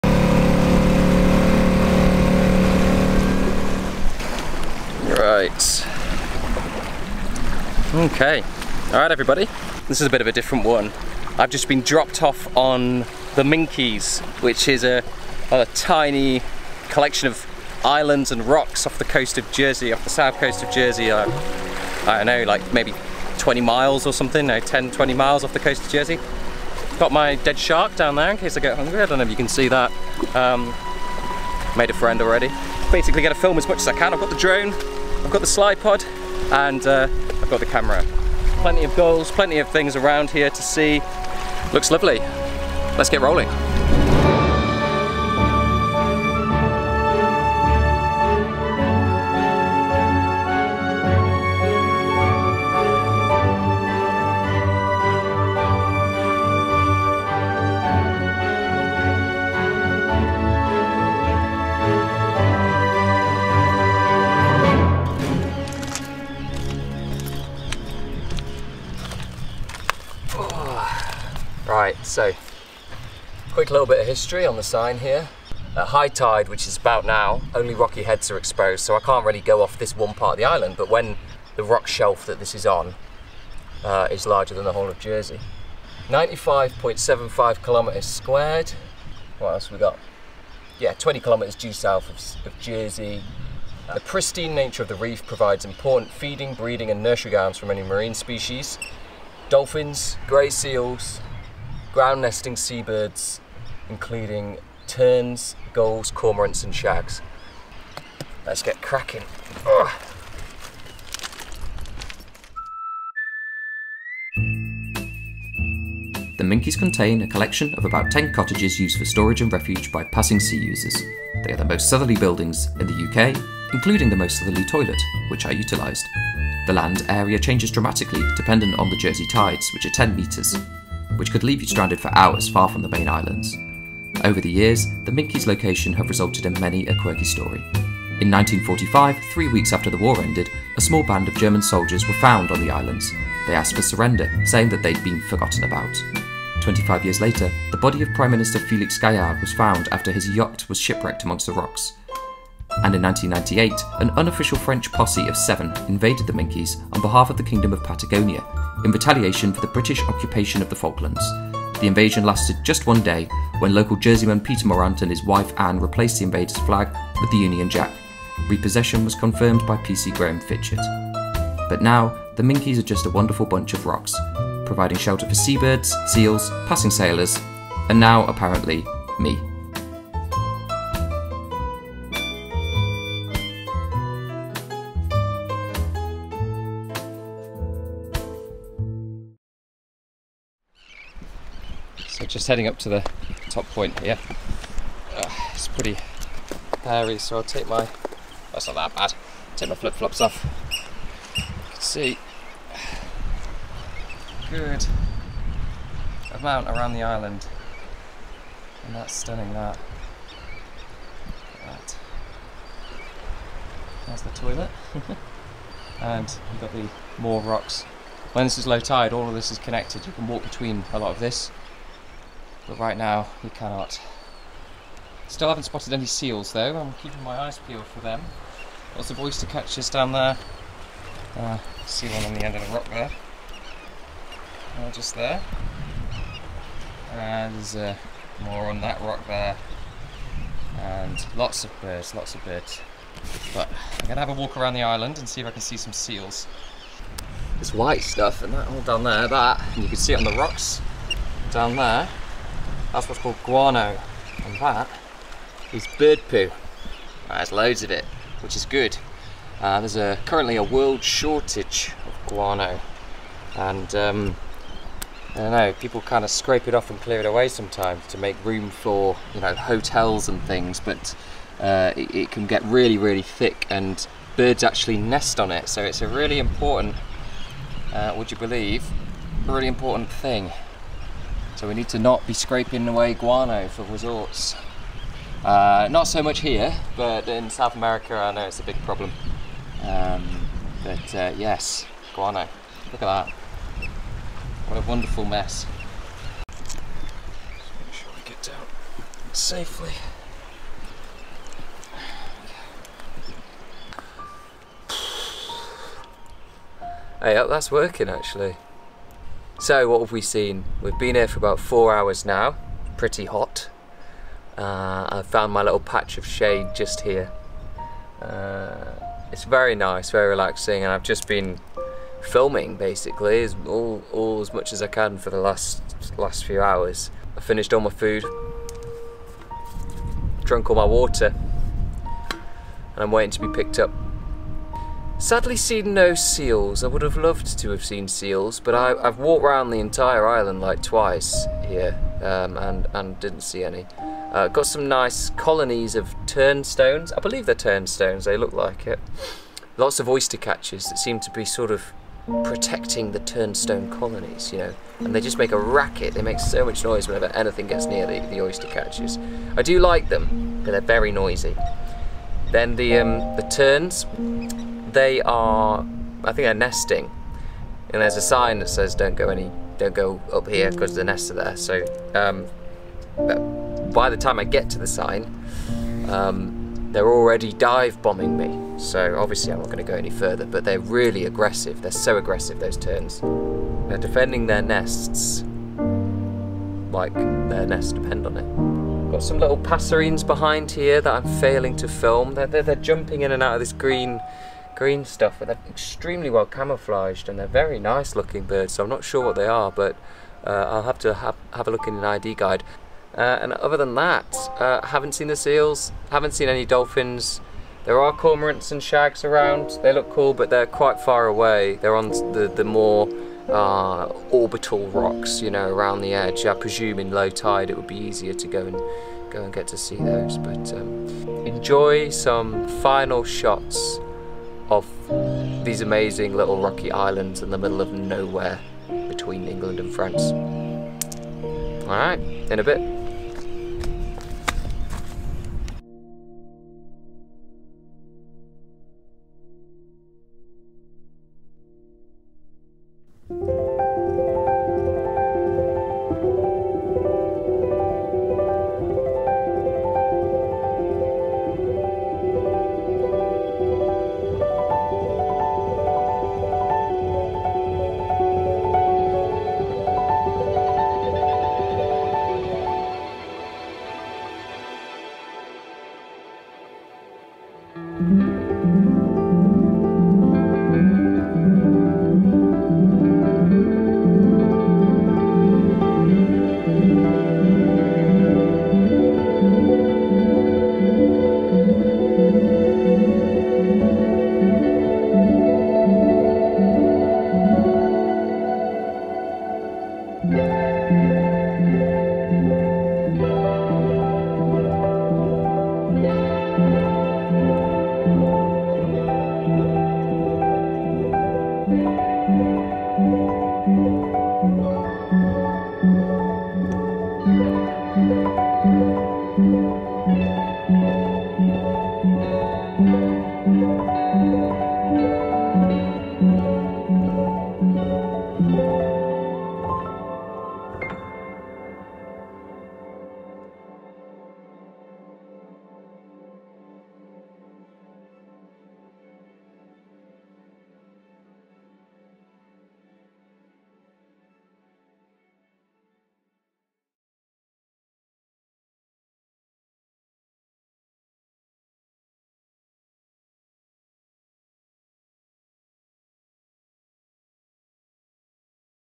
Exploring the British Isles' most southerly point
I got dropped off on the Minkies (Les Minquiers) and had 5 hours to explore the most southerly point in the British Isles.

This was filmed back in August 2022, on my old kit, and young brain.

Filmed on Panasonic Lumix GH5, Mavic Air 2S

Follow - @mattjarvismedia